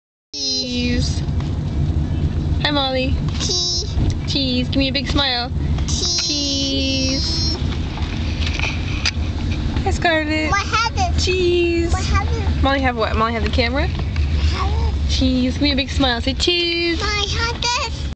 GIMME THE DAMN CAMERA MOM!
Mali trying to make me give her the camera ("Mali have this! Mali have this!")